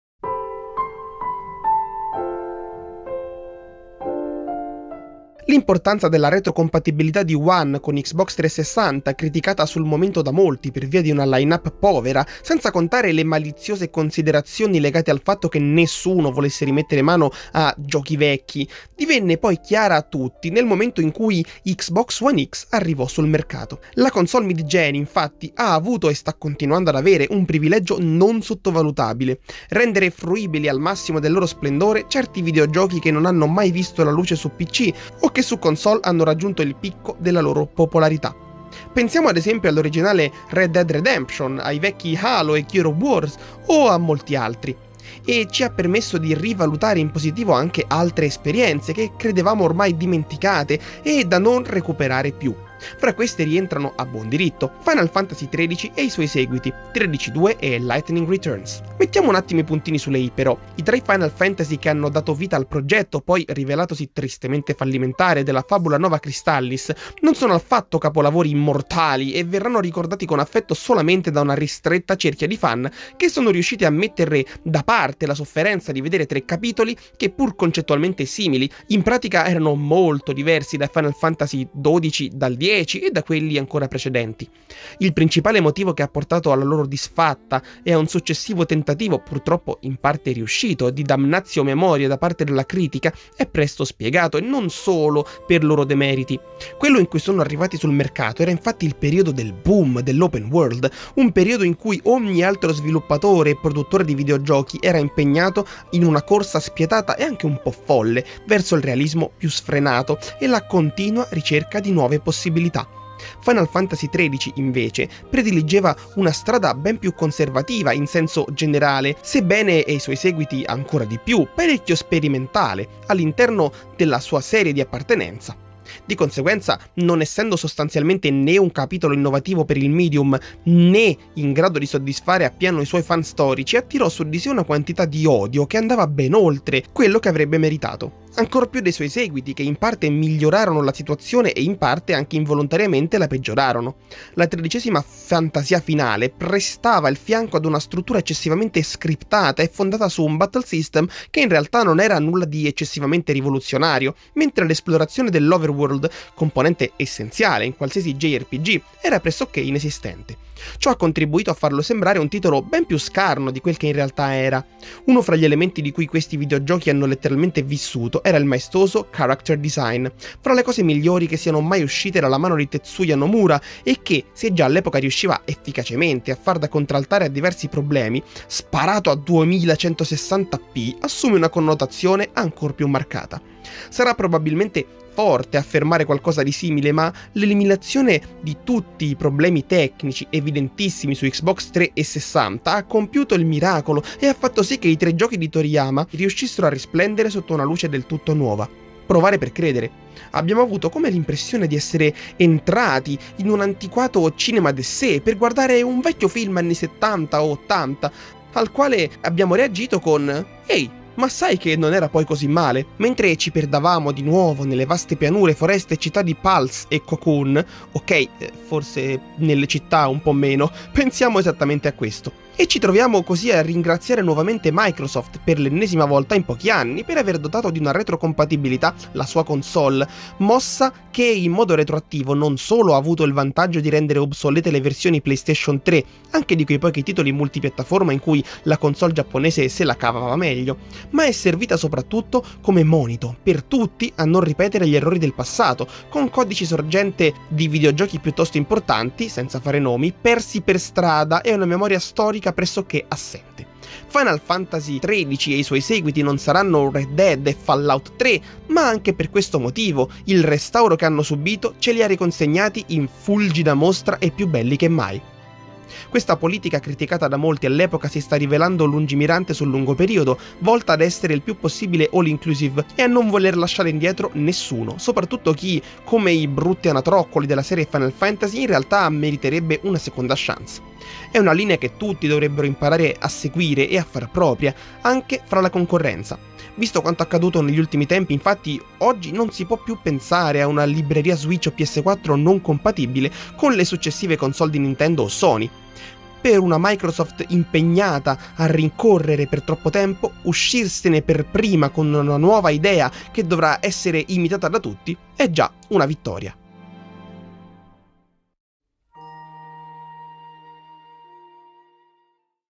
Final Fantasy XIII e seguiti risorgono su Xbox One | Editoriale
Final Fantasy XIII e i suoi due seguiti approdano su Xbox One, più belli che mai, grazie alla retrocompatibilità.